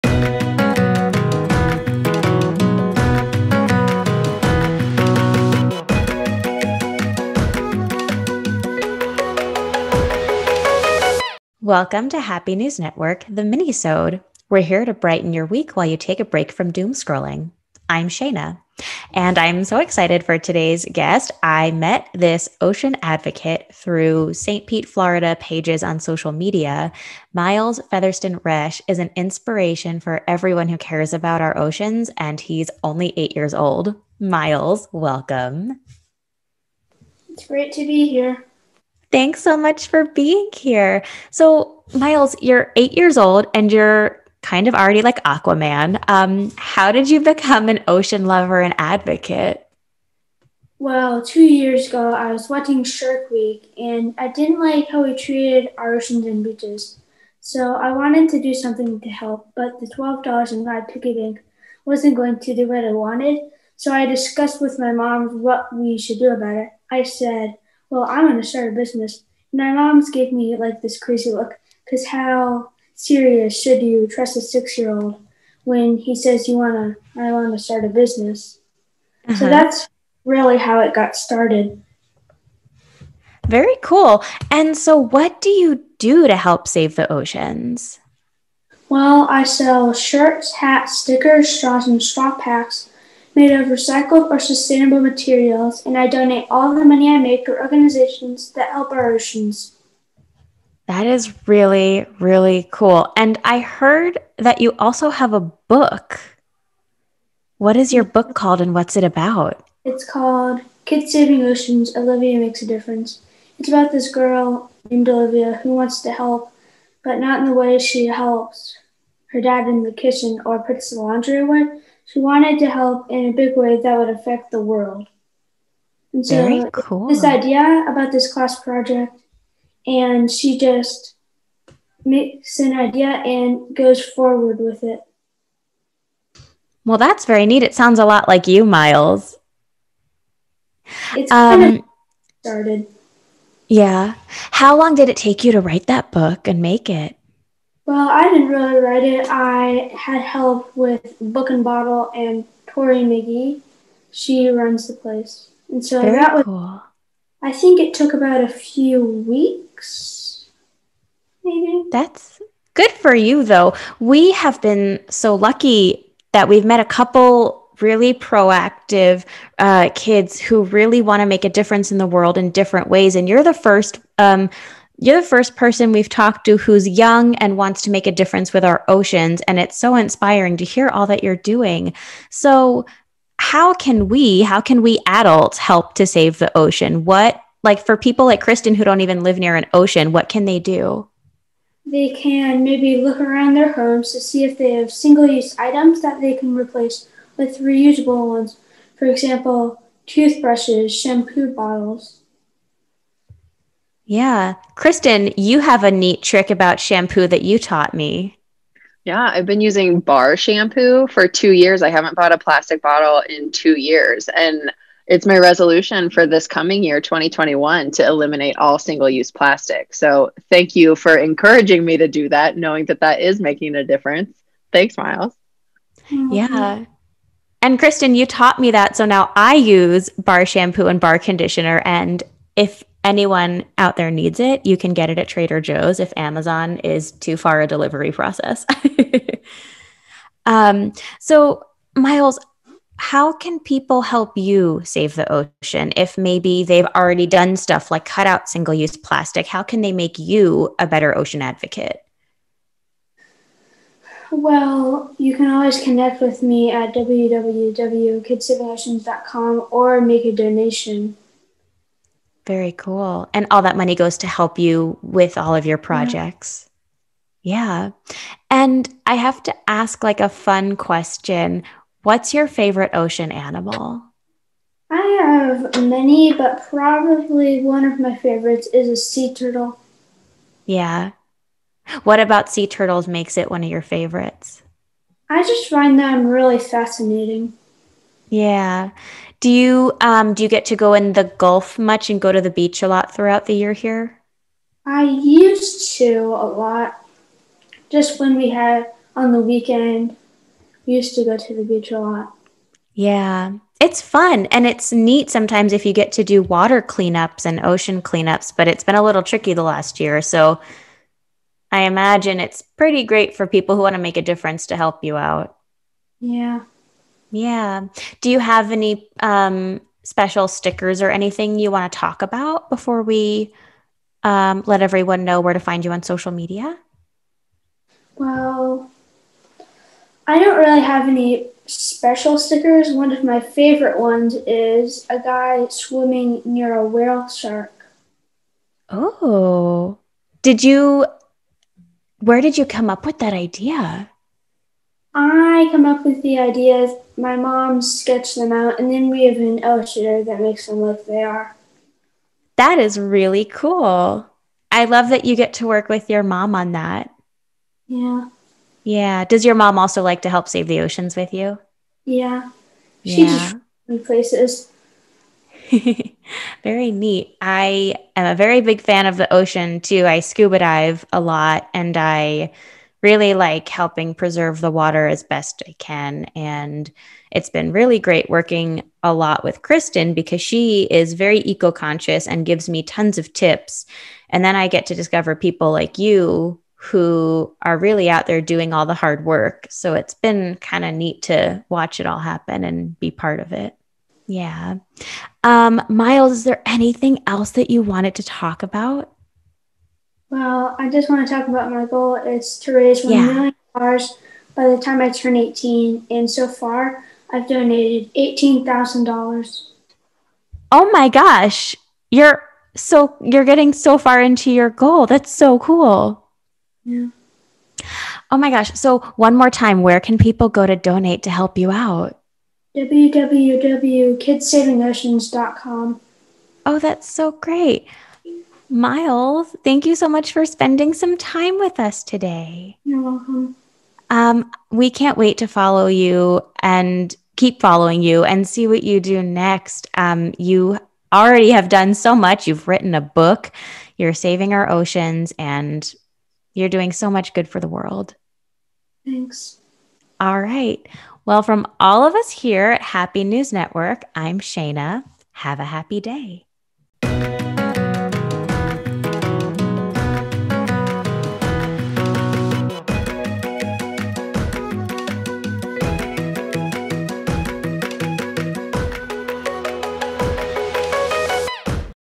S2 Minisode 6: Miles Fetherston-Resch
Join us as we chat with Miles Fetherston-Resch, creator of Kids Saving Oceans!